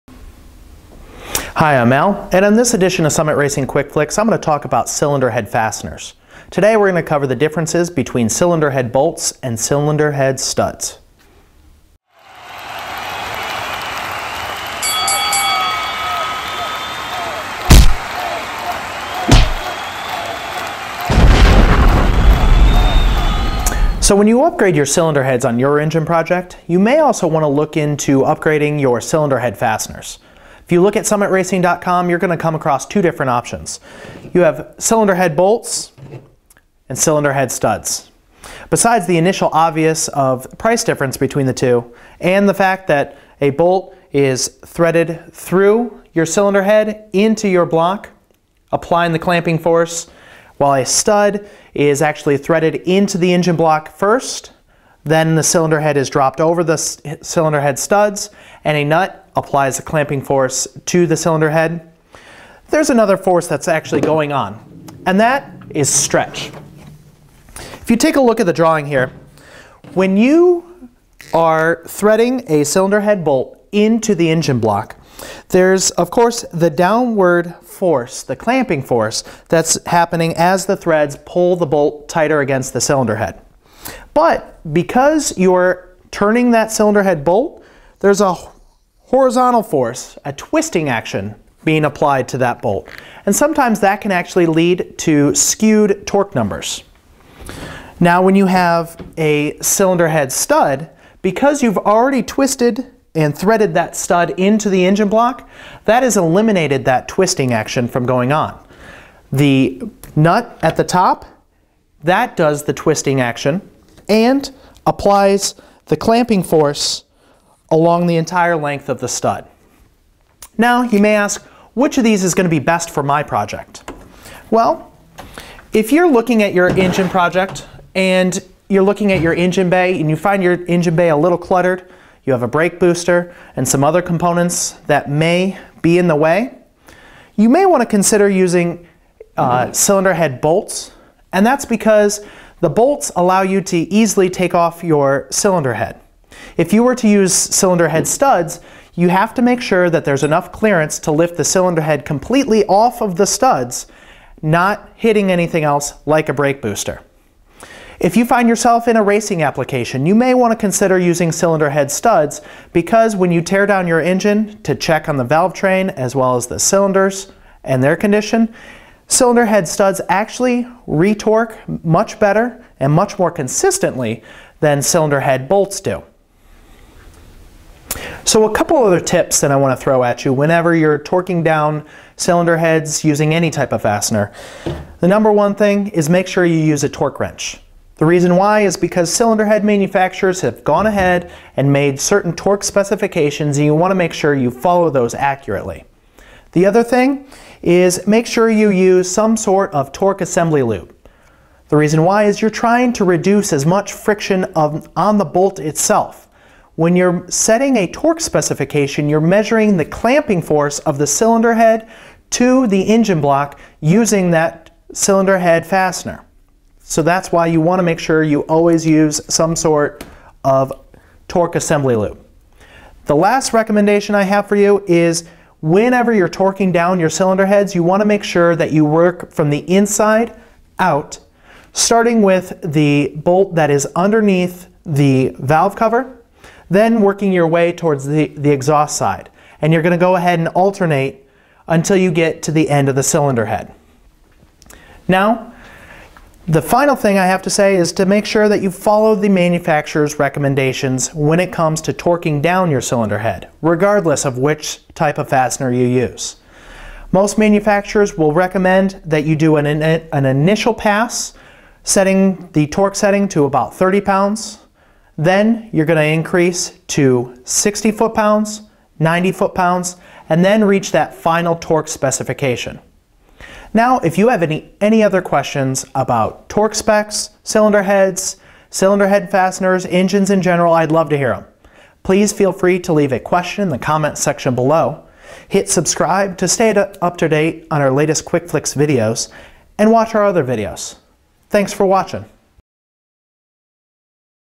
Cylinder Head Bolts vs Cylinder Head Studs - Summit Racing Quick Flicks
Learn the difference between cylinder head bolts and cylinder head studs and which is better for your vehicle. This video also provides tips for properly installing your cylinder head fasteners. For Summit Racing's selection of fasteners,